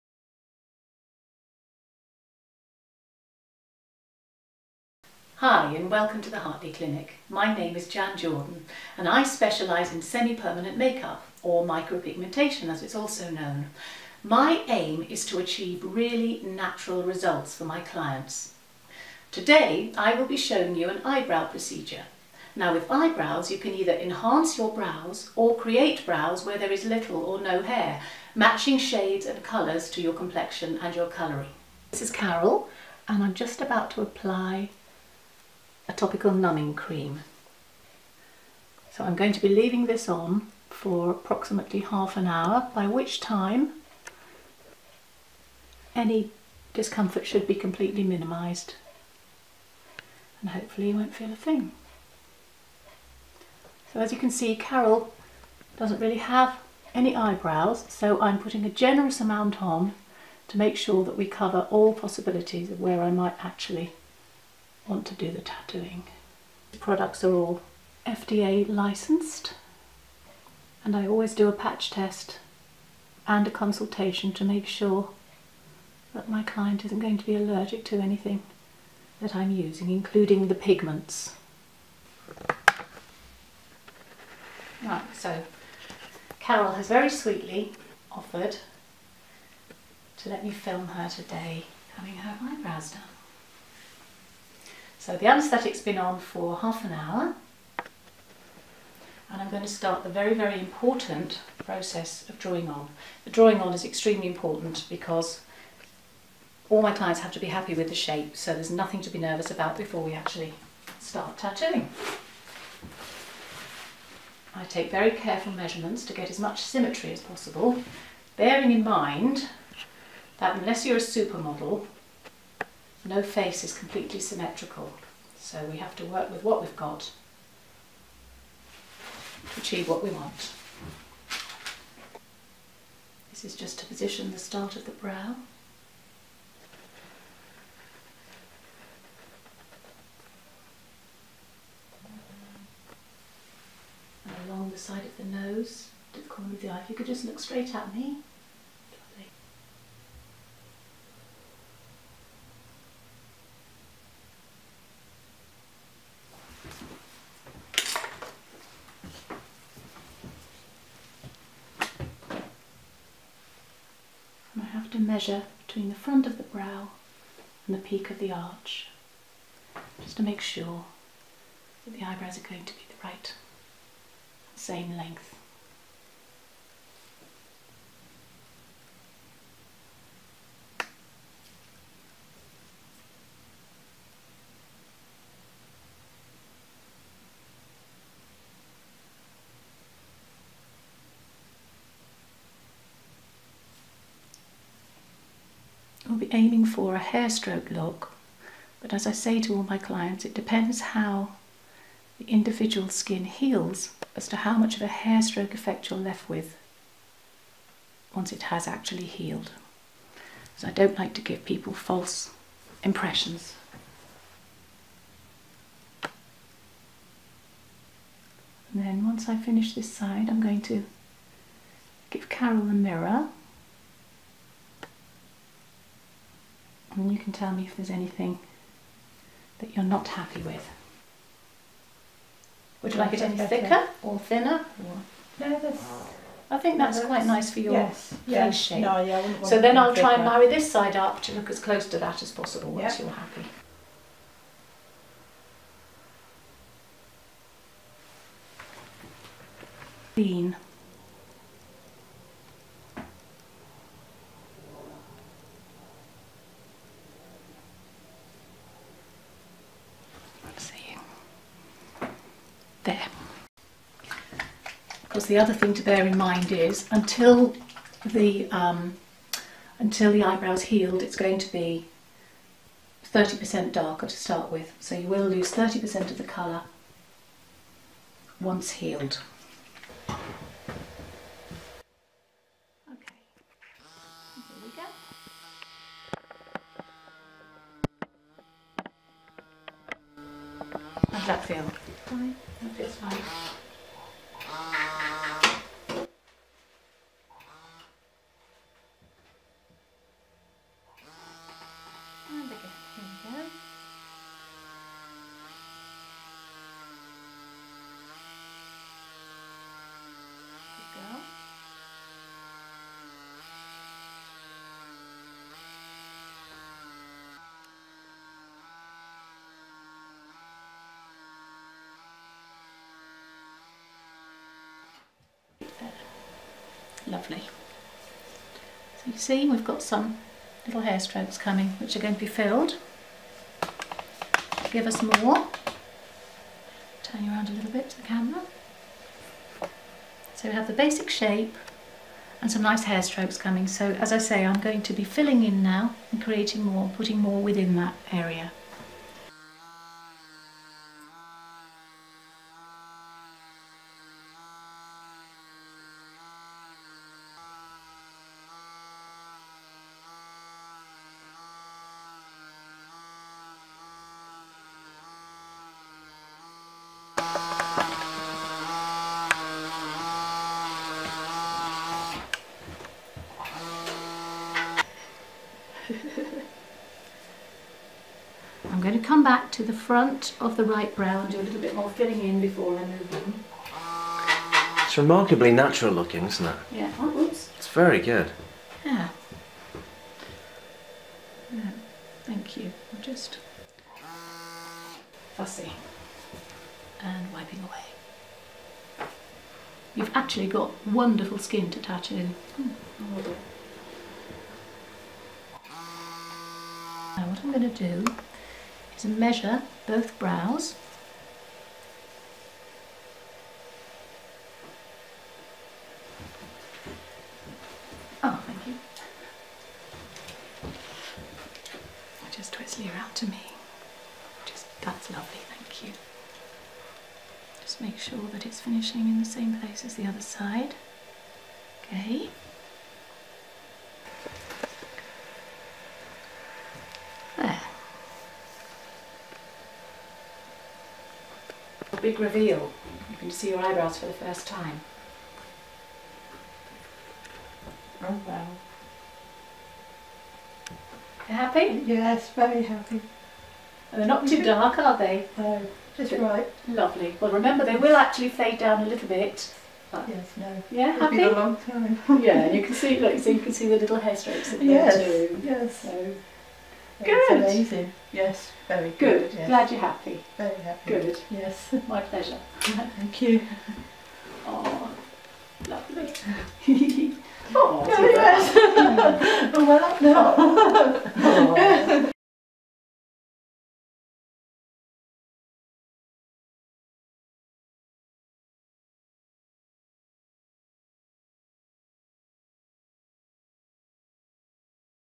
Semi Permanent Makeup London @ Jan Jordan
Semi-Permanent Makeup & Micropigmentation Specialist in Eyebrows, Lips, Eyeliner tattooing. Beautifully natural results.

 Micropigmentation, or Semi Permanent Makeup, is a tattoo process that delivers organic pigment into the epidermal layers and the first "Reticular" dermal layer of the skin using very fine needles. The treatment is semi permanent because we all, eventually, shed our epidermal skin layers, leaving a faint trace of pigment in the reticular layer. People shed the epidermal layers at different rates, meaning that fading of colour varies from person to person. The colour of the tattooed area will seem darker than expected until healed, which is about 5-7 days, when the colour fades by about 30%, leaving a lovely, soft, natural result.